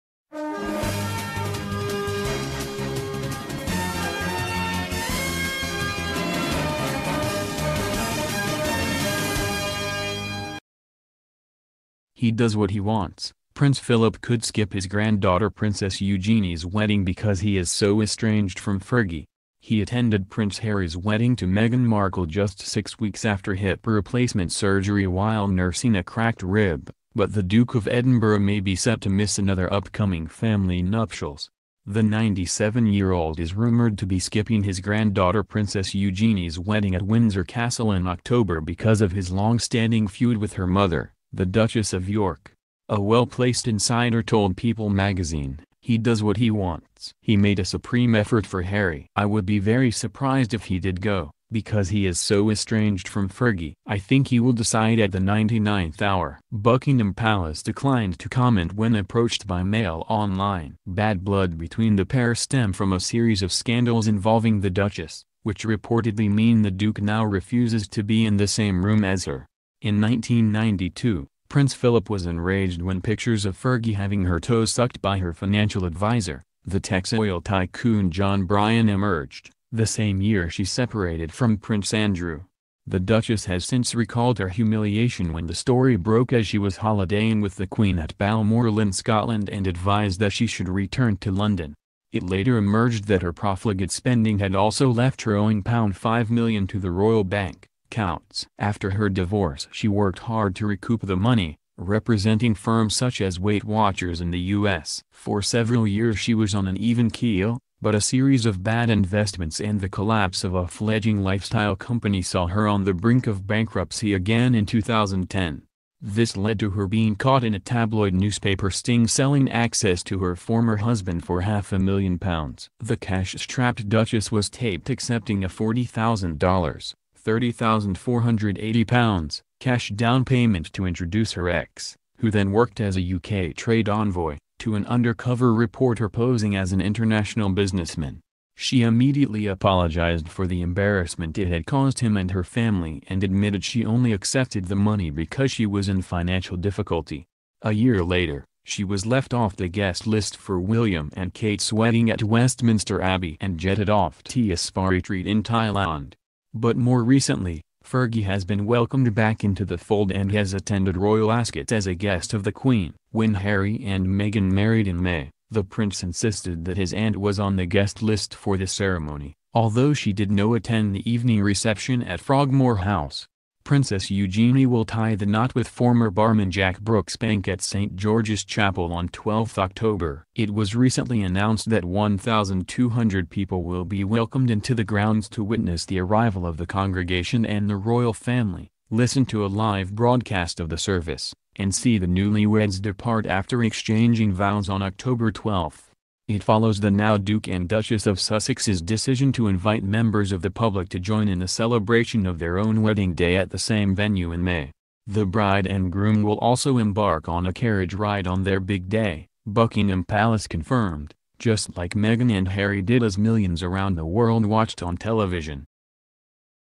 Prince Phillip could skip his granddaughter Princess Eugenie's wedding because feud with Fergie
'He does what he wants' Prince Phillip could skip his granddaughter Princess Eugenie's wedding because 'he is so estranged from Fergie'.